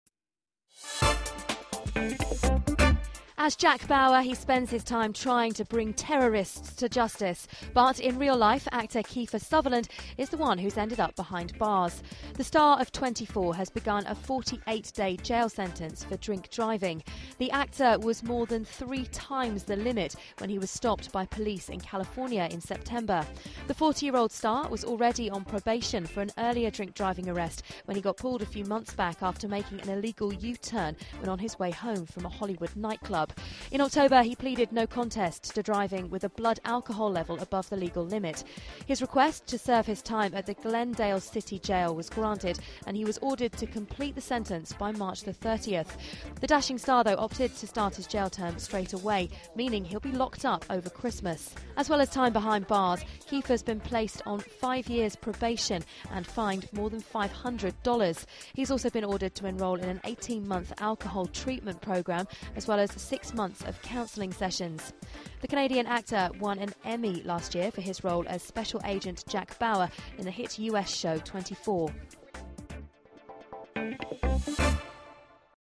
Kiefer Sutherland behind bars in jail
Hollywood actor Kiefer Sutherland has started a 48 day jail sentence for drink driving in Los Angeles. Test